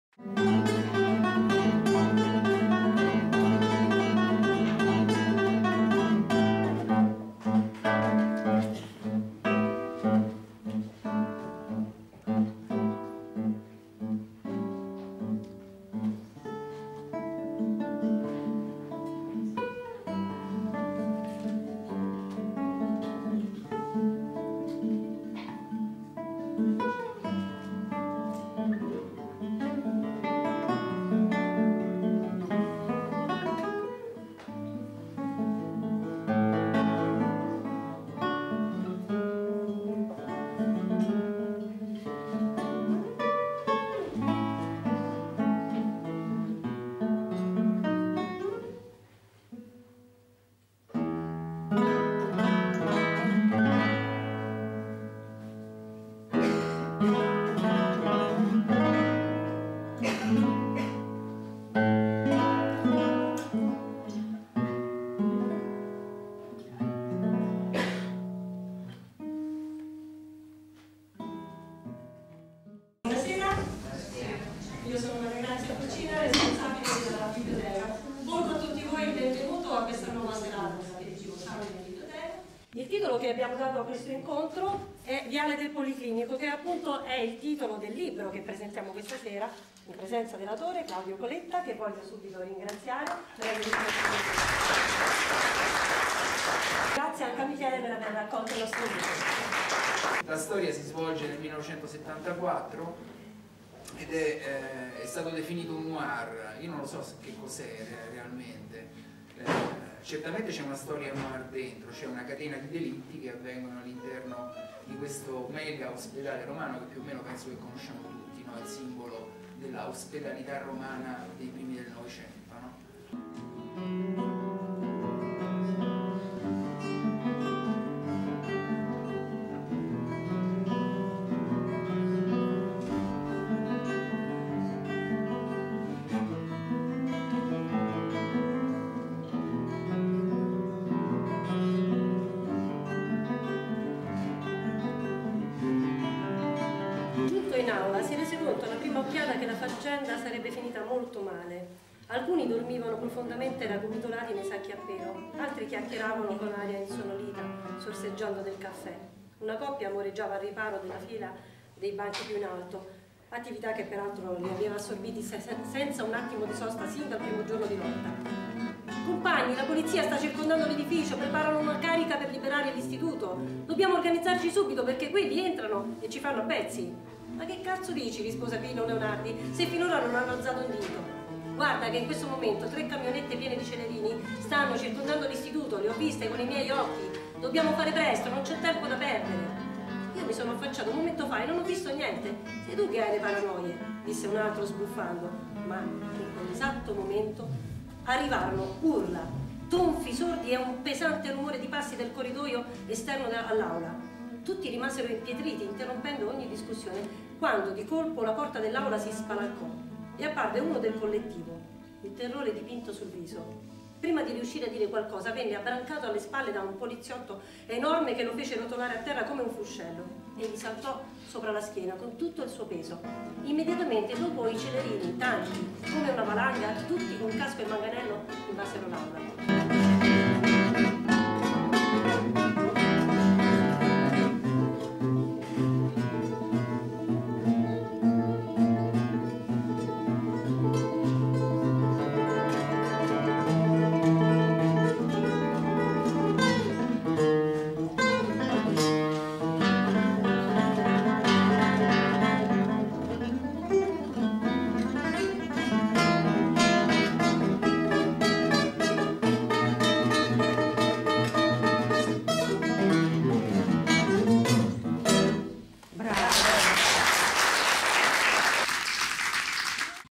APERITIVO SOUND IN BIBLIOTECA 2012 "VIALE DEL POLICLINICO"
Venerdì 18 Maggio 2012 ore 19.00 -Presentazione del libro "Viale del Policlinico" di Claudio Coletta, Sellerio editore Palermo, Intrattenimento musicale a cura del chitarrista Michele Di Filippo